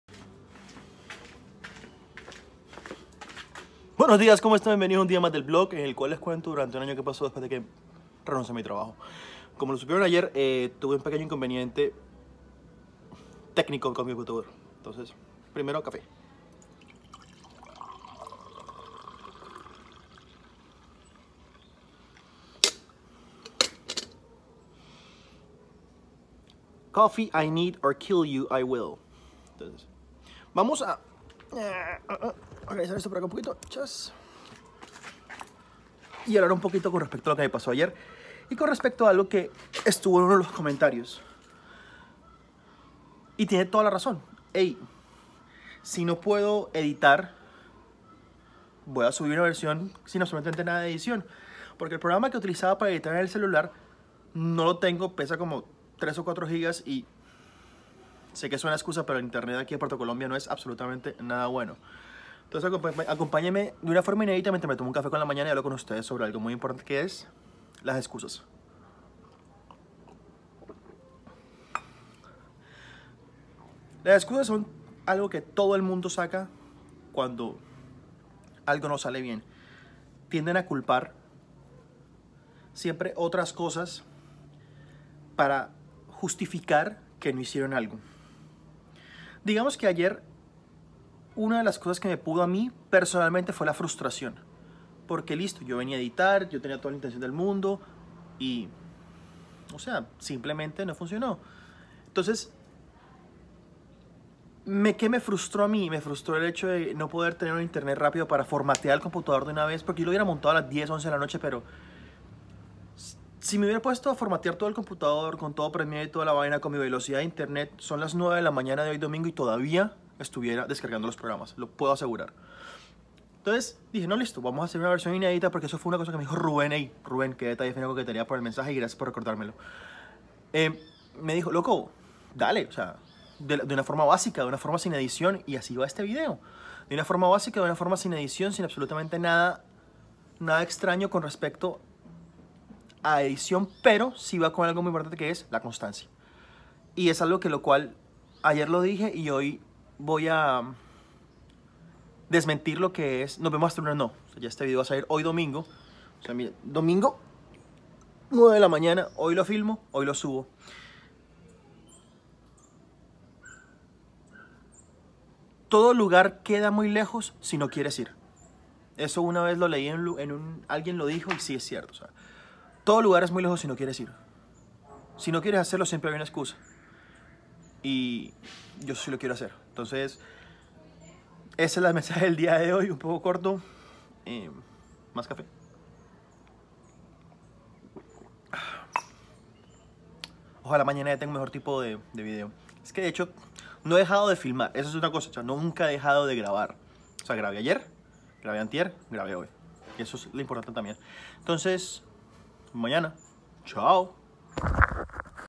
hoy sin nada de edición
Después de un percance de computador y que un amigo me recordara que no hay excusas, una versión sin edición del Vlog de hoy